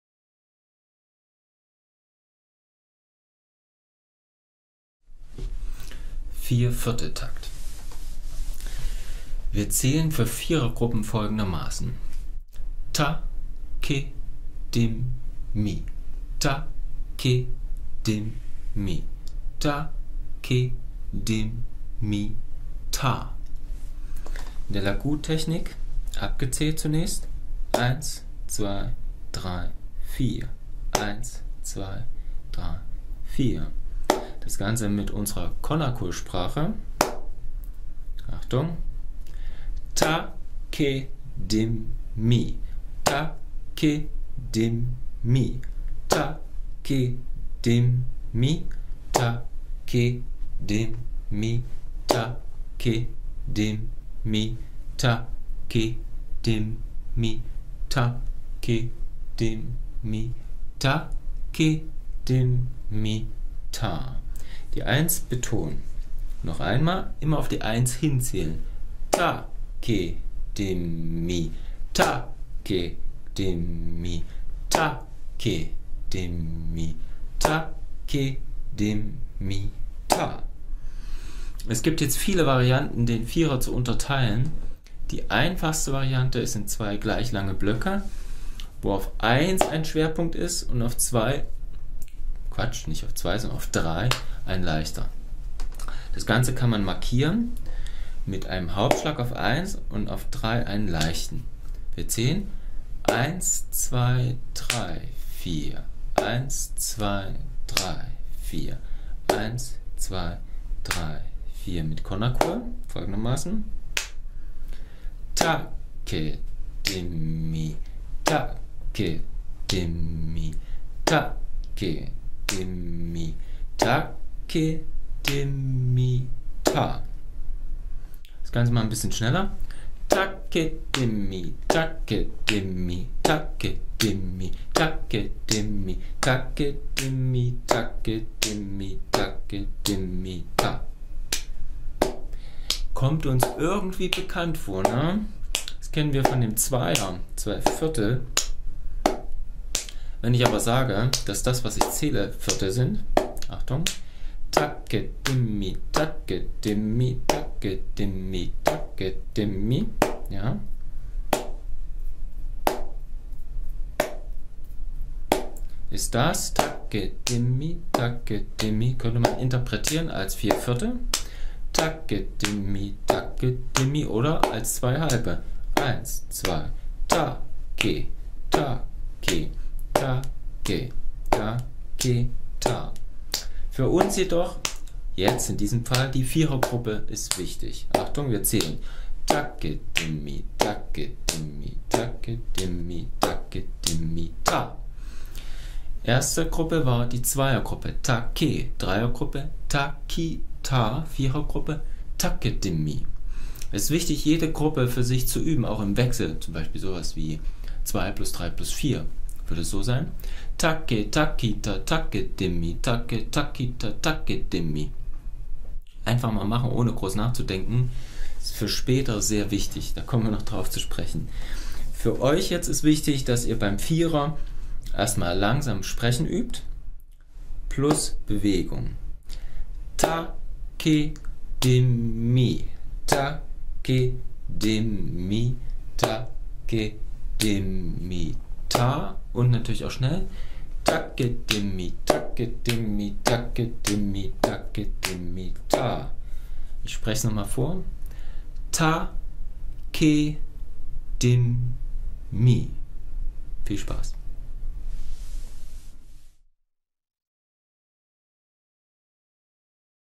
Master the 4/4 TIME SIGNATURE! How to REALLY clap and count!
Ich stelle den 4/4 Takt vor. Wieder gib's Konnakol und Handgeste dazu.
This is part of the book: CAJON - The Beginning, by Torsten Pfeffer. Start right away with the book